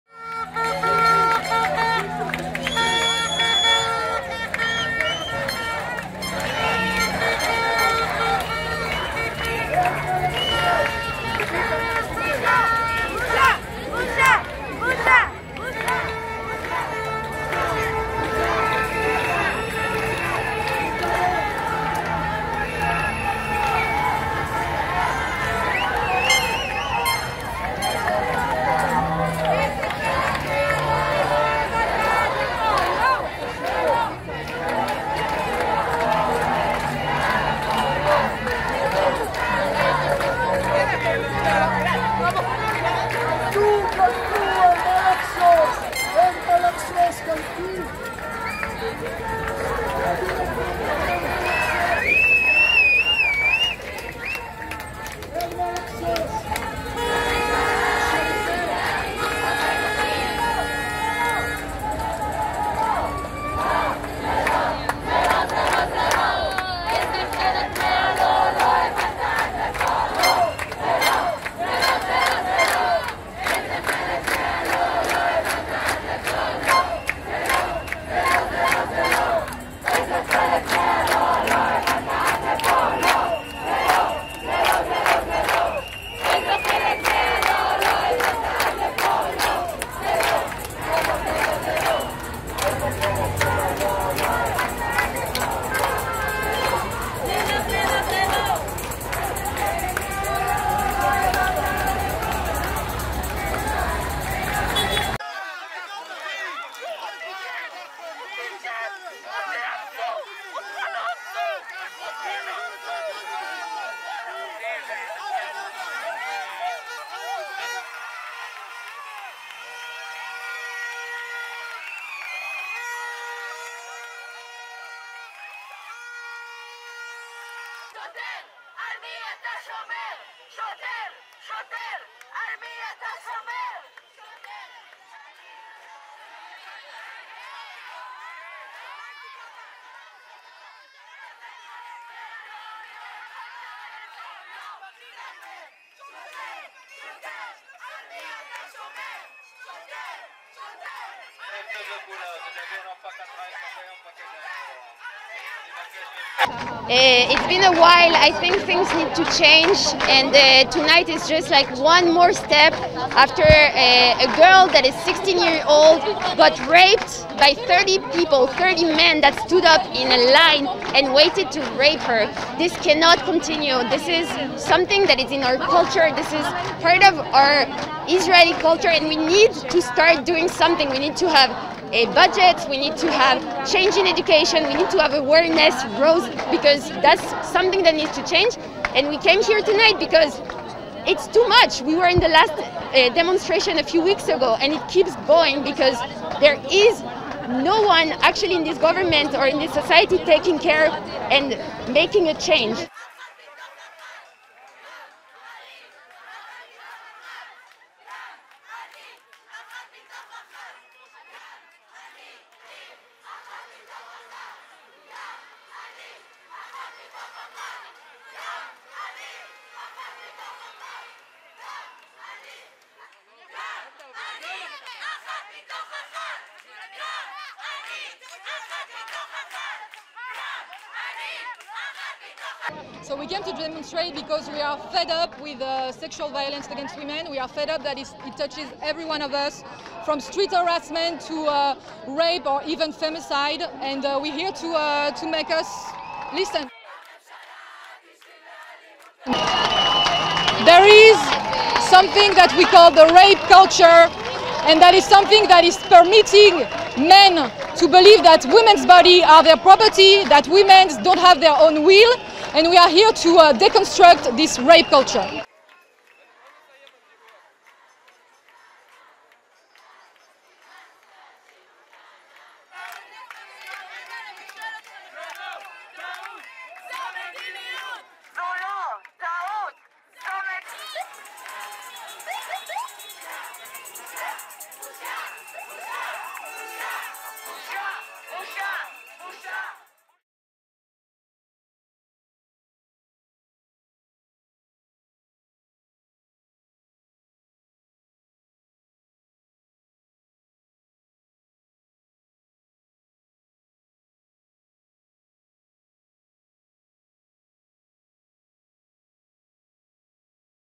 Israel : Hundreds protest suspected gang rape of 16 year old girl.
Hundreds of demonstrators took the streets of Tel Aviv on Thursday to decry sexual violence and stand in solidarity with a 16-year-old girl suspected of being a victim of a gang rape at a hotel in Eilat.

Demonstrators were seen marching, chanting and carrying signs, as some of them, visibly angry faced off with police officers.

"We came to demonstrate because we are fed up with sexual violence against women, we are fed up that it touches everyone of us, from street harassment to rape or even femicide and we are here to make us listen," said protester, Ilana Witzman.
According to reports, two people have been detained in connection to the suspected rape attack, with one of the suspects denying the rape allegations but stating that more than 30 people were involved in the episode.